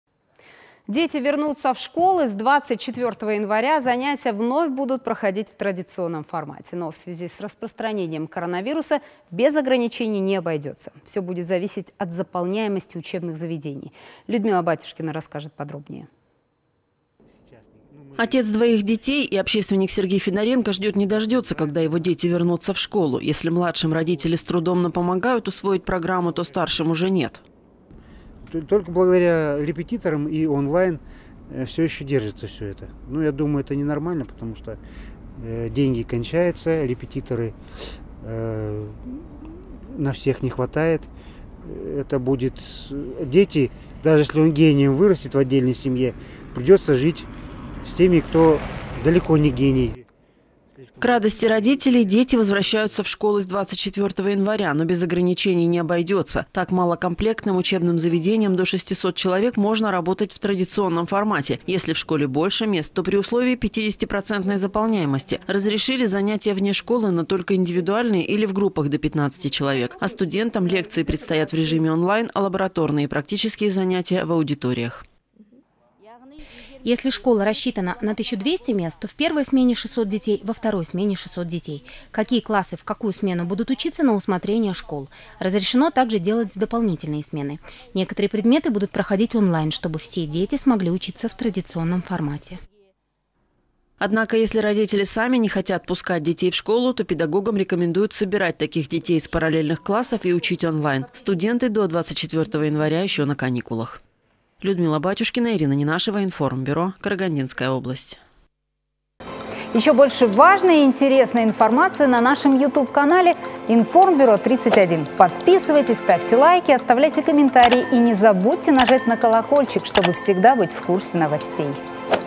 Снова дистанционка? Как будут учиться школьники и студенты
Дети вернутся в школы. С 24 января занятия вновь будут проходить в традиционном формате.  Но в связи с распространением коронавируса без ограничений не обойдется, все будет зависеть от заполняемости учебных заведений.
Отец двоих детей и общественник Сергей Федоренко ждет не дождется, когда его дети вернутся в школу. Если младшим родители с трудом, но помогают усвоить программу, то старшим уже нет.
СЕРГЕЙ ФЕДОРЕНКО, родитель, общественный деятель:
– Только благодаря репетиторам и онлайн все держится. Но это ненормально. Деньги кончаются, репетиторов на всех не хватает. Это будут дети… Даже если он гением вырастет в отдельной семье, придется жить с теми, кто далеко не гений.
К радости родителей, дети возвращаются в школы с 24 января. Но без ограничений не обойдется. Так, малокомплектным учебным заведениям до 600 человек можно работать в традиционном формате. Если в школе больше мест, то при условии 50-процентной заполняемости. Разрешили занятия вне школы, но только индивидуальные или в группах до 15 человек. А студентам лекции предстоят в режиме онлайн, а лабораторные и практические занятия – в аудиториях.
САПУРА ТУЛЕКОВА, заместитель председателя Комитета дошкольного и среднего образования МОН РК:
– Если школа рассчитана на 1 200 мест, то в первой смене 600 детей, во второй смене 600 детей. Какие классы в какую смену будут учиться – на усмотрение школ. Разрешено также делать дополнительные смены. Некоторые предметы будут проходить онлайн, чтобы все дети смогли учиться в традиционном формате.
Однако если родители сами не хотят пускать детей в школу, то педагогам рекомендуют собирать таких детей из параллельных классов и учить онлайн. Студенты до 24 января еще на каникулах.